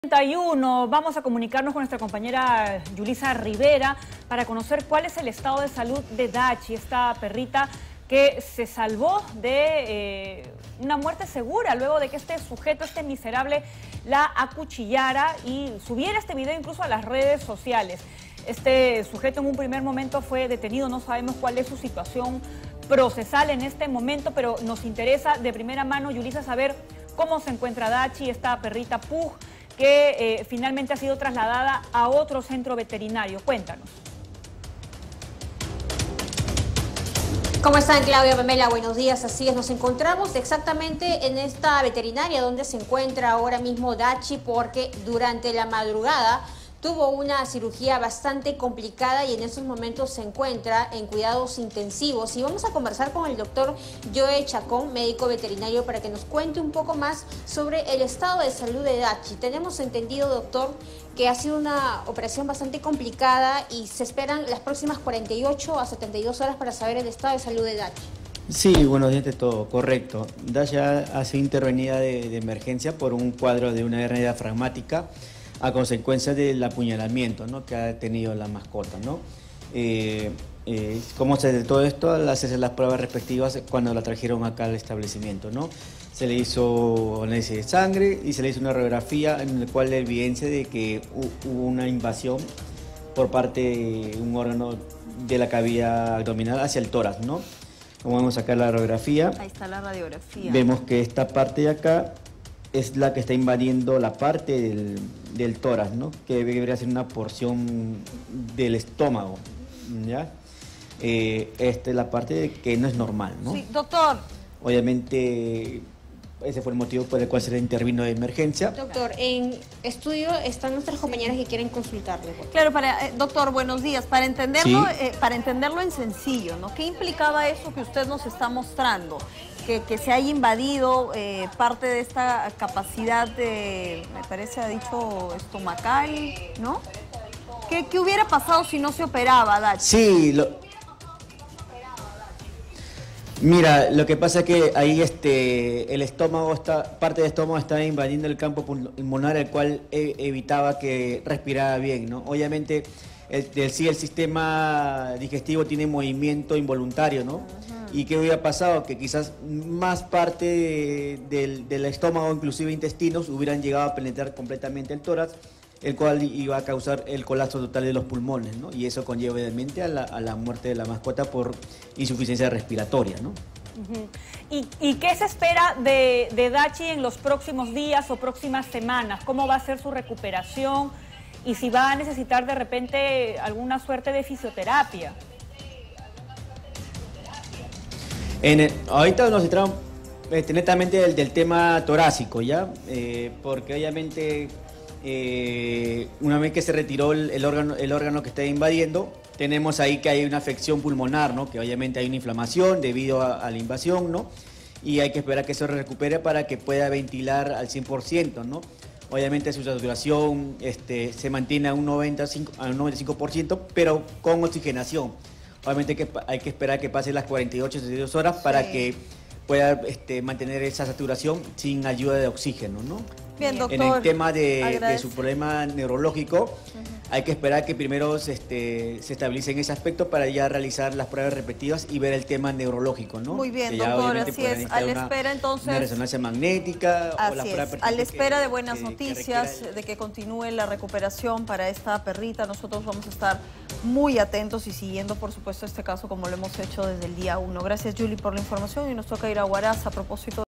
Perrita “Dachi” fue operada con éxito y se espera su recuperación
Se ha hecho tendencia por la indignación que ha levantado este nuevo caso de maltrato animal. "Dachi", la perrita que fue acuchillada en el distrito de La Victoria se espera que pueda recuperarse luego de haber sido intervenida de emergencia.

Como se recuerda el pasado 27 de agosto, un sujeto identificado como Alonso Santa Cruz Túpac Yupanqui acuchilló a la mascota de su expareja sentimental, luego se supo que el agresor tiene antecedentes policiales por robo y violencia sexual. La agresión contra la perrita de raza pug sucedió en una transmisión en vivo por Facebook.

En un primer momento la mascota de dos años se encontraba internada en la clínica veterinaria Prevet, sin embargo, en horas de la madrugada del 28 de agosto fue retirada pese a su delicado estado de salud a solicitud de la dueña de la mascota, quien se encuentra en Estados Unidos y fue trasladada a la veterinaria Animal Surco.

En un enlace un vivo con Buenos Días Perú, el responsable de la intervención de la can herida, el doctor veterinario Joel Chacón indicó que “Dachi fue intervenida de emergencia por el apuñalamiento que fue víctima (…) parte de su estomago dañado invadía la parte pulmonar impidiendo que respirara bien, de no haber sido intervenida hubiera muerto por insuficiencia respiratoria”.

Cabe señalar que tras la complicada intervención se espera que en las próximas 48 a 72 horas para saber si su estado de salud ha mejorado y ya puede respirar con normalidad.